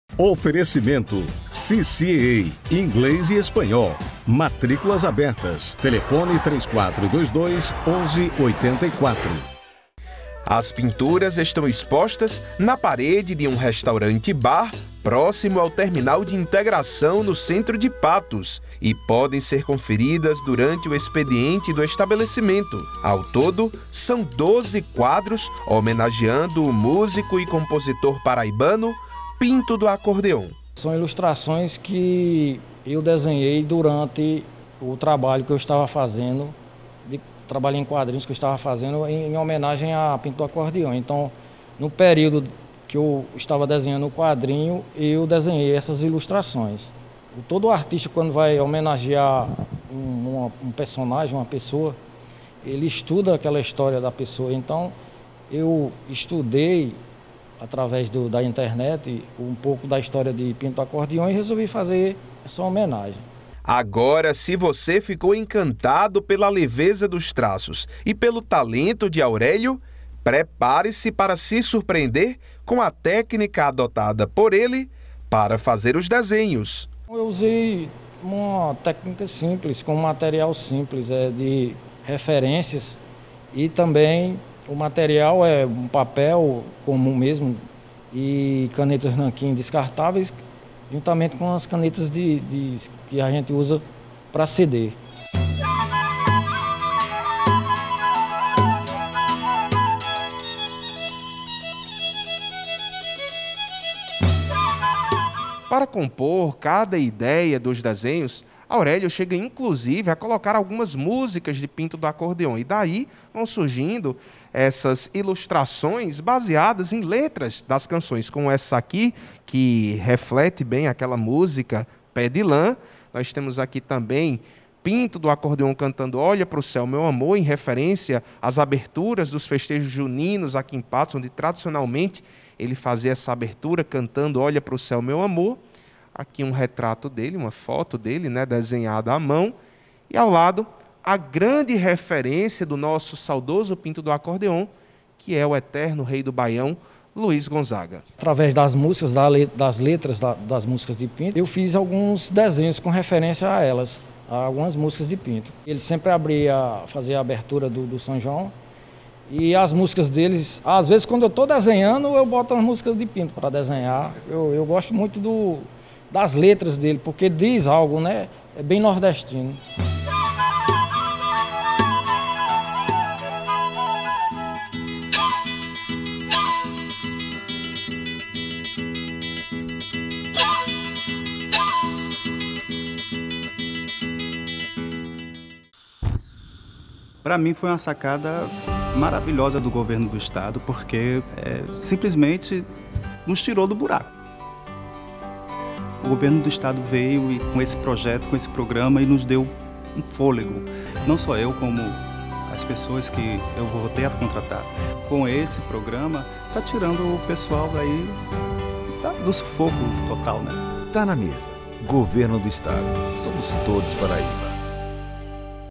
EXPOSIÇÃO HOMENAGEIA PINTO DO ACORDEON
Desenhista expõe trabalhos feitos com caneta em restaurante, no centro de Patos